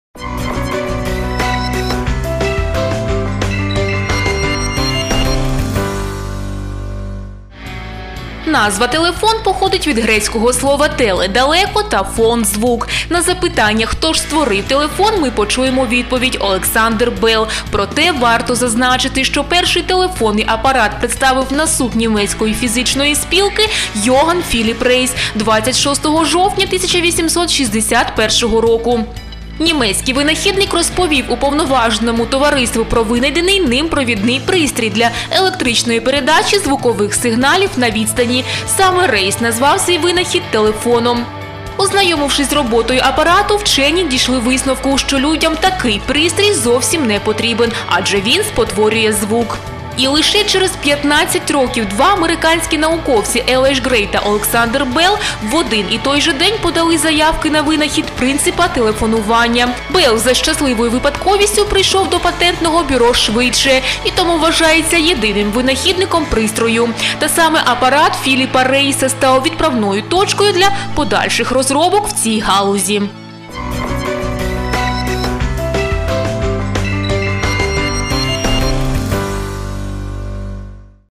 Подія дня. Телефон
26 жовтня 1861 року німецький винахідник Філіпп Рейс у Франкфурті продемонстрував свій винахід, який  назвав телефоном.